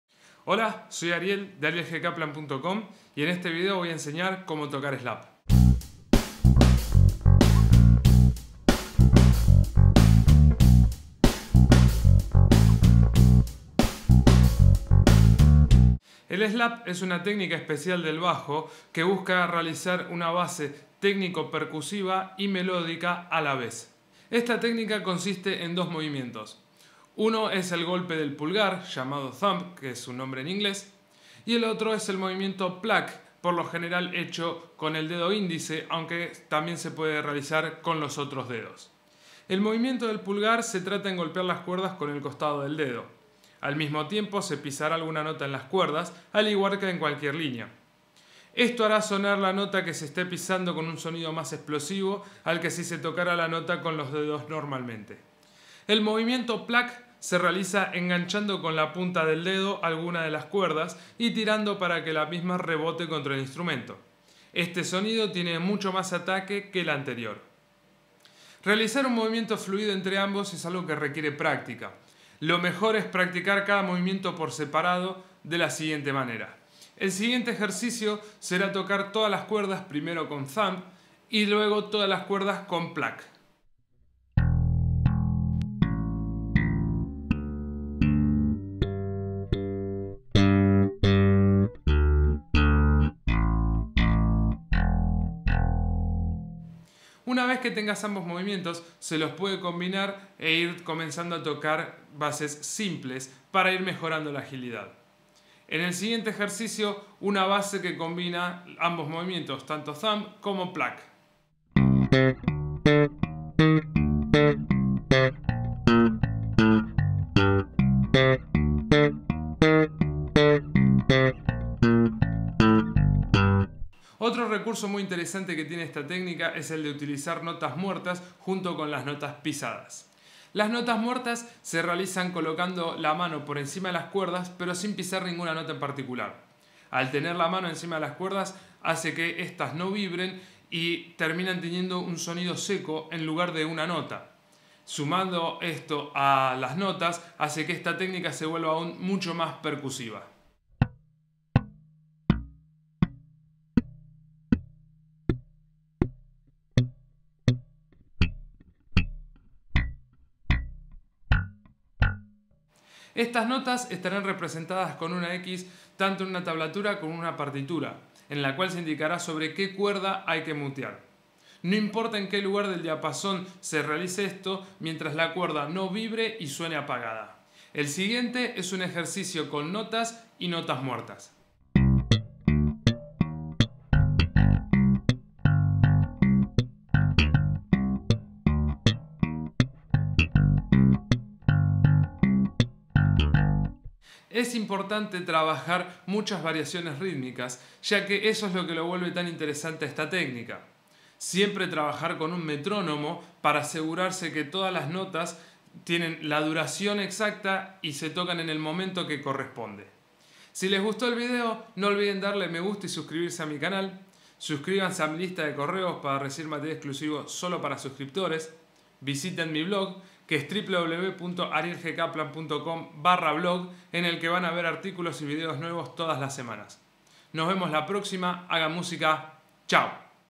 Como tocar slap - Ariel Gustavo Kaplan
En este video enseño a tocar slap. Los movimientos correctos que hay que realizar en la mano derecha para que puedas tocar esta técnica.